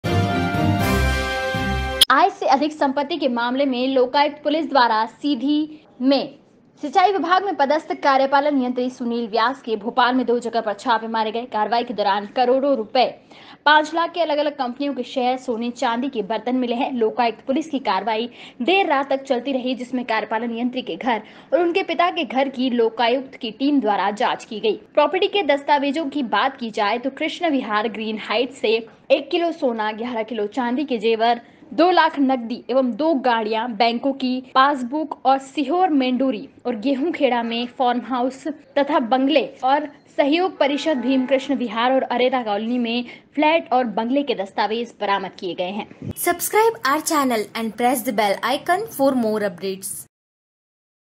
इंजीनियर सोने और चांदी के बर्तनों में खाता था खाना | Lokayukta Raids Bhopal Engineer's Residence
करोड़ों की संपत्ति मिली छापे में

लोकायुक्त की टीम ने सीधी जिले के सिंचाई विभाग के एक्जीक्यूटिव इंजीनियर सुनील व्यास के भोपाल और सीधी स्थित ठिकानों पर छापा मारा। इंजीनियर के घर में कार्रवाई के दौरान सोने और चांदी के बर्तन मिले हैं। बताया गया है कि इंजीनियर इन्हीं बर्तनों में खाना खाता है। इसके अलावा करोड़ों की प्रॉपर्टी और करीब 5 लाख के अलग-अलग कंपनियों के शेयर मिले हैं।
Lokayukta Raid at Engineer's House

 Lokayukta Raids Bhopal Engineer's Residence